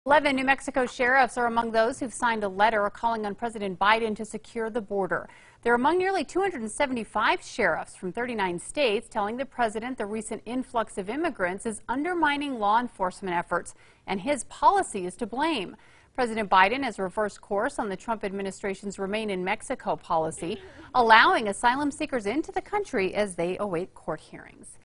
11 New Mexico sheriffs join letter calling out Biden's border policy
They are among nearly 275 sheriffs from 39 states telling the president the recent influx of immigrants is undermining law enforcement efforts and his policy is the blame. President Biden has reserved course on the Trump administration's 'remain in Mexico' policy, allowing asylum-seekers into the country as they await court hearings.